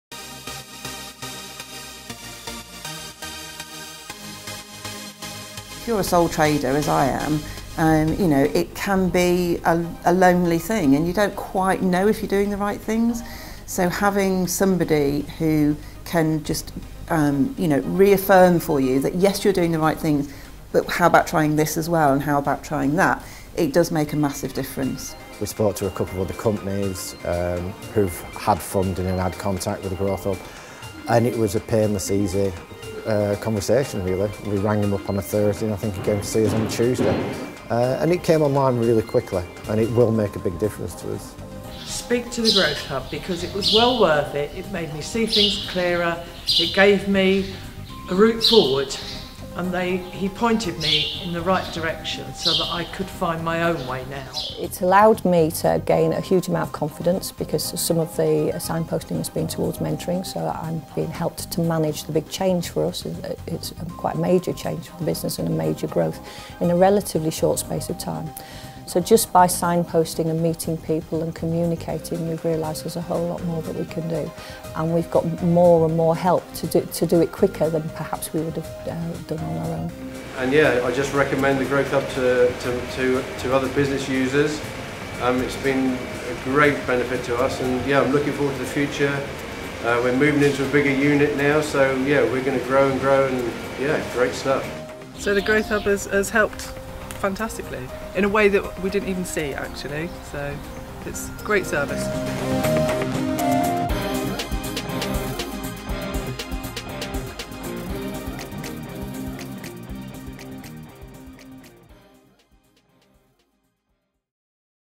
The Cornwall and Isles of Scilly Growth & Skills Hub
What is the Cornwall and Isles of Scilly Growth & Skills Hub? Who can we help and how?